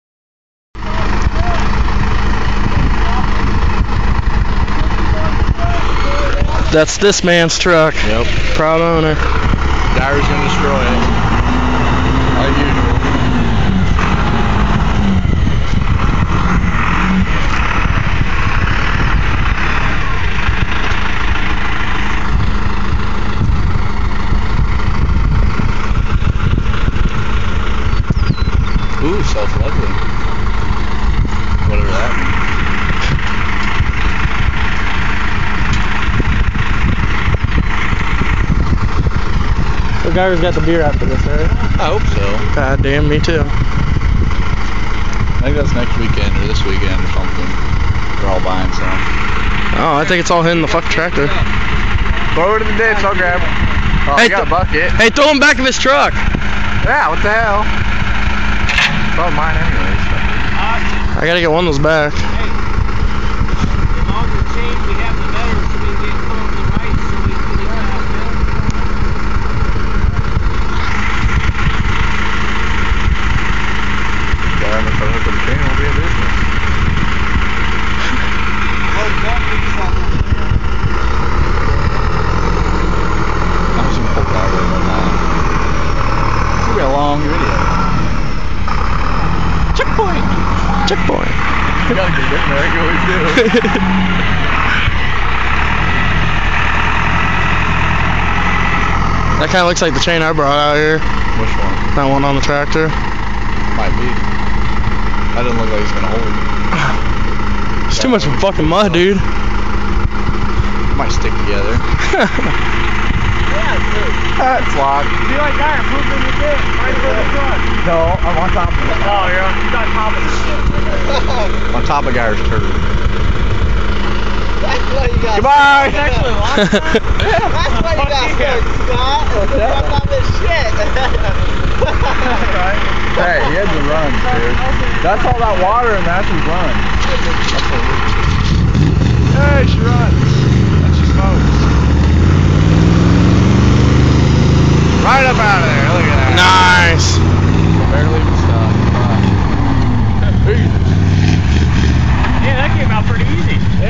Scotts truck getting pulled out
Scotts truck stuck in mud getting pulled out by tractor